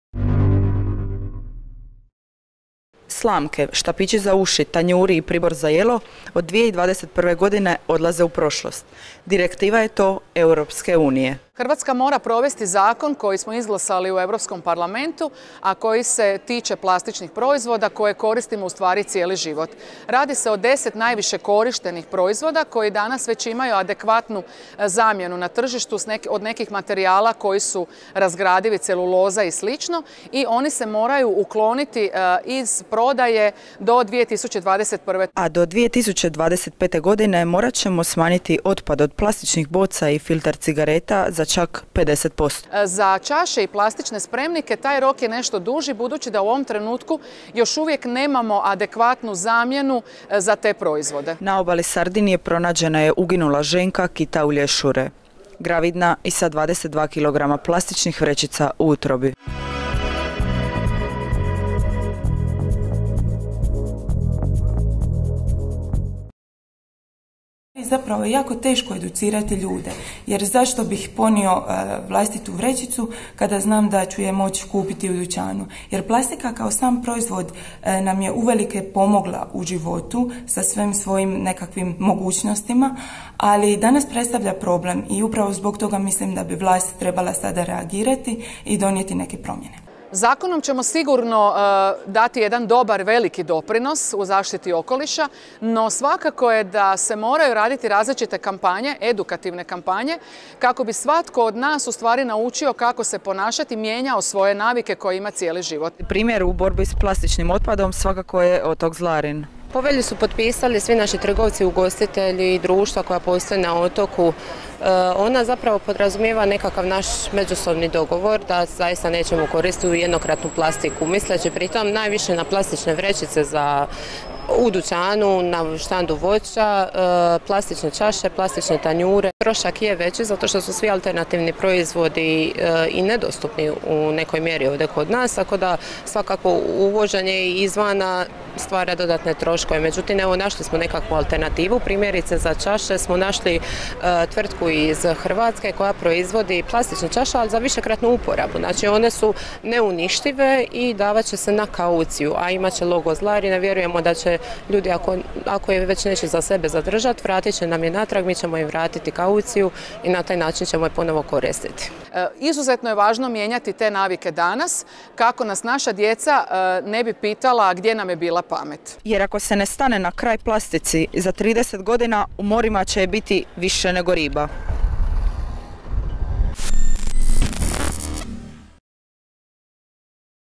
Za dvije godine stiže zabrana jednokratne plastike!
Zastupnici Europskog parlamenta prošli tjedan podržali su zabranu jednokratnih plastičnih proizvoda na tržištu EU, veće recikliranje plastičnih boca te smanjenje filtera za cigarete koji sadrže plastiku, i to za 50 posto do 2025. godine. A do 2021. godine bit će zabranjena proizvodnja slamki, štapića za uši, pribora za jelo, tanjura, čaša od ekspandiranog polistirena te štapića za pridržavanje balona, koji zajedno s izgubljenom opremom za ribolov čine 70 posto otpada na europskim obalama i u morima.

Europarlamentarka Biljana Borzan, magistrica biologije i ekologije mora Ivana Marin te direktorica Turističke zajednice Zlarin Katarina Gregov, govorile su o tom problemu za ŠIBaj TV!

Novinarka: Ana-Marija Crnica